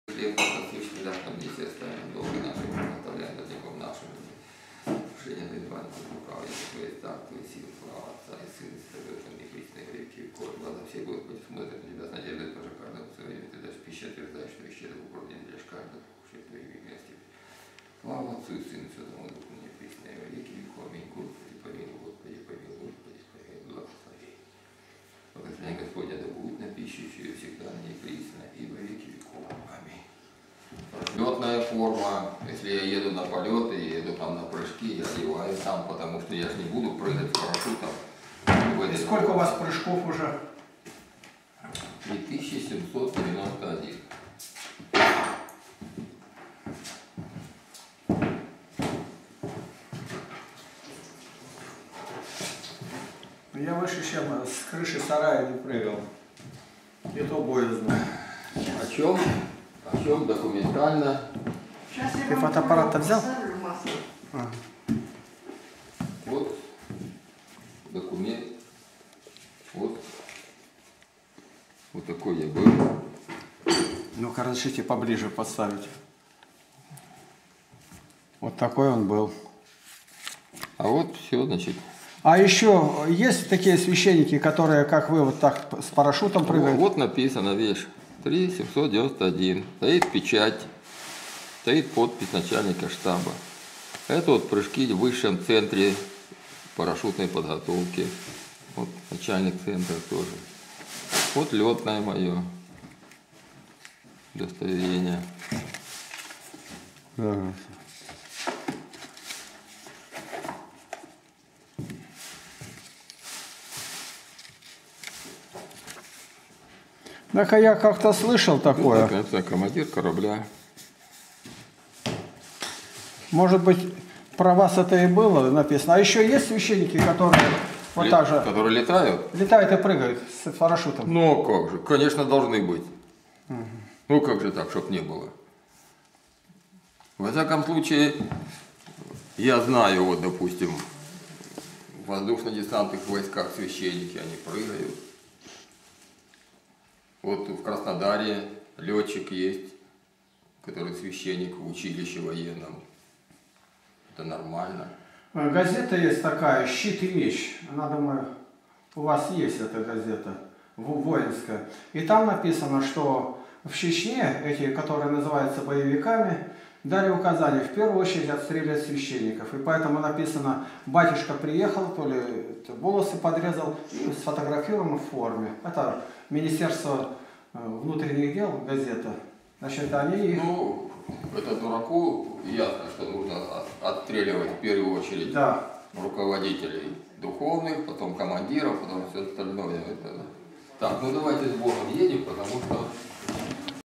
2013-07-10 - Завтрак у о Игоря Бобрикова. Республика Адыгея. в гостях Игнатий Лапкин(377,378)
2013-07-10 - Завтрак у о Игоря Бобрикова. Республика Адыгея. в гостях Игнатий Лапкин